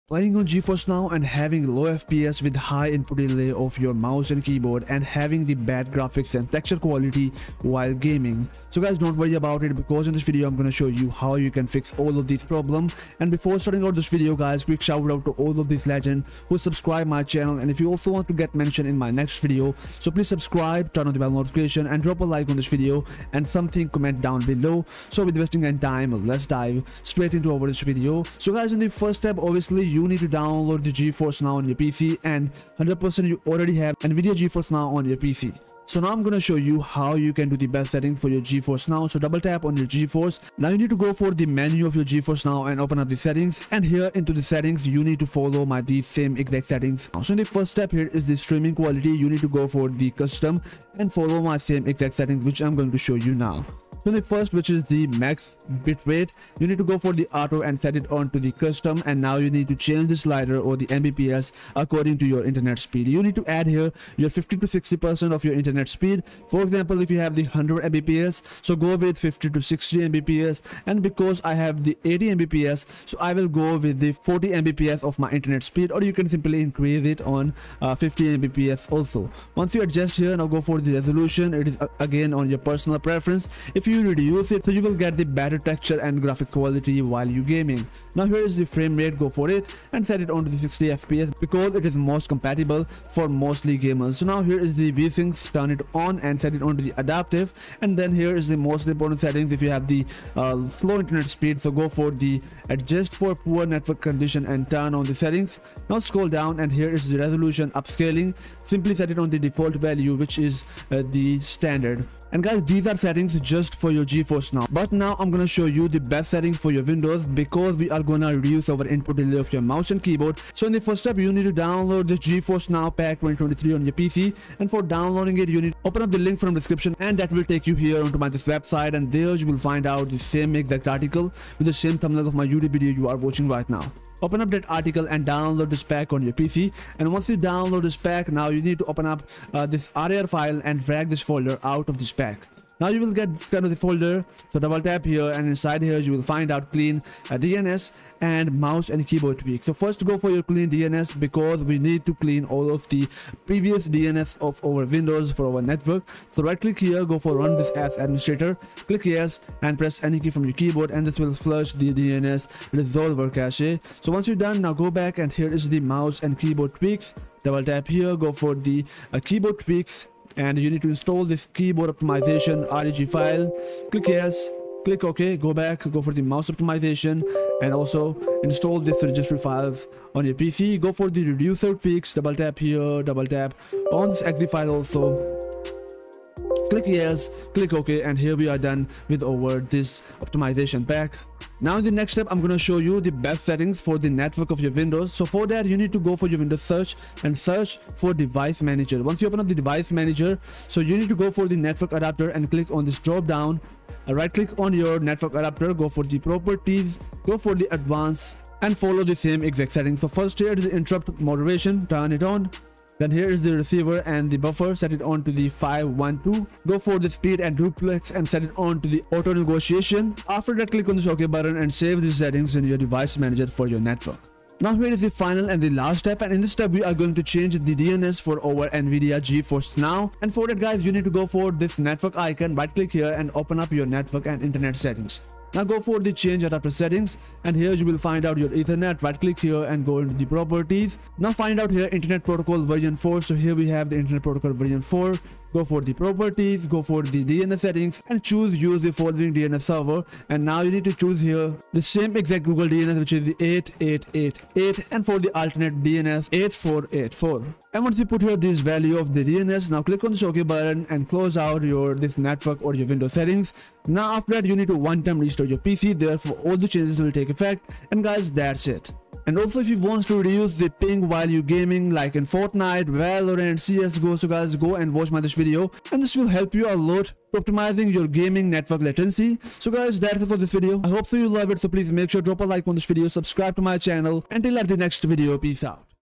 GeForce Now BEST SETTINGS : BOOST FPS & ZERO INPUT DELAY IN GEFORCE NOW!
In this video, I will guide you GeForce Now best settings and GeForce Now fps boost guide, and important Windows settings to get zero input delay in GeForce Now. Make sure to watch the complete video to do geforce now best settings for bad internet.

THESE TWEAKS WORK FOR WINDOWS 11 AS WELL 🔥

Timestamps 🕛
00:00 - Intro
00:28 - GeForce Now Best Settings
01:55 - GeForce Now Zero Input Delay (Optimization Pack)
03:11 - Windows Network Settings
04:46 - End